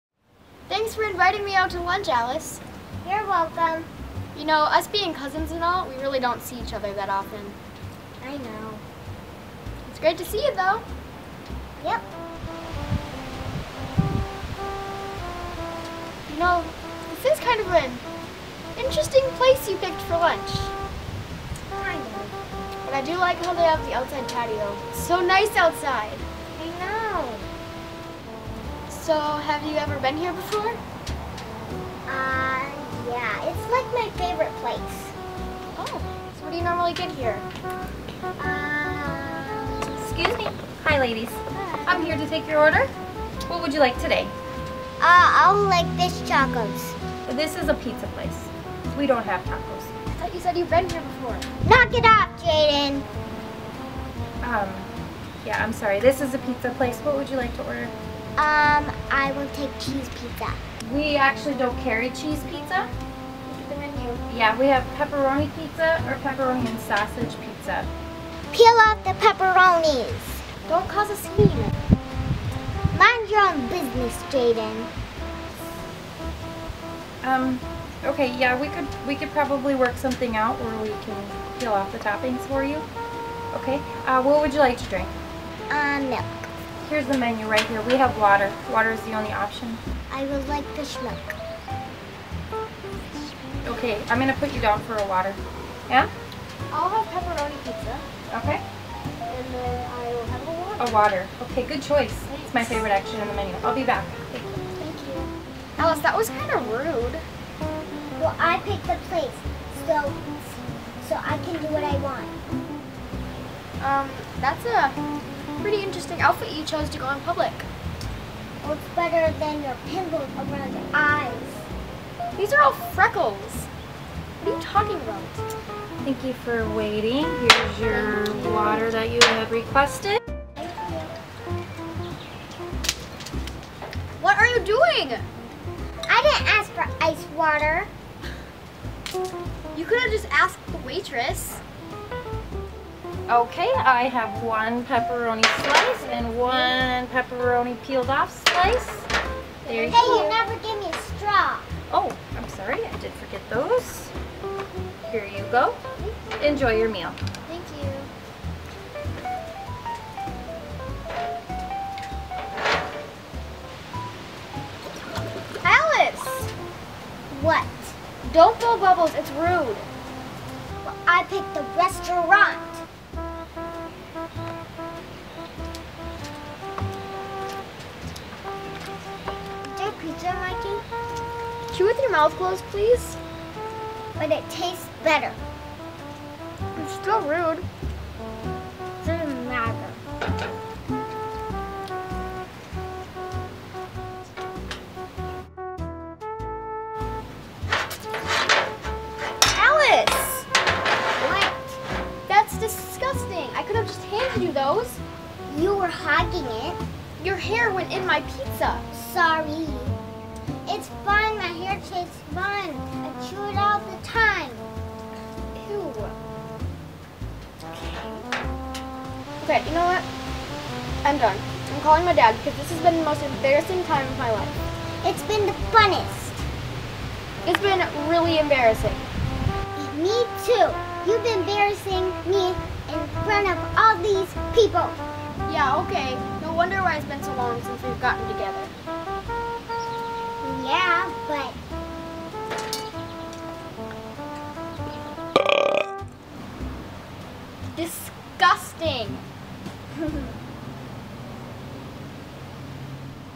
Jaidyn and Her EMBARRASSING Cousin Alice!!
An oldie but goodie!  Enjoy this throwback skit of Jaidyn and her sister, Lyla, as her embarrassing cousin!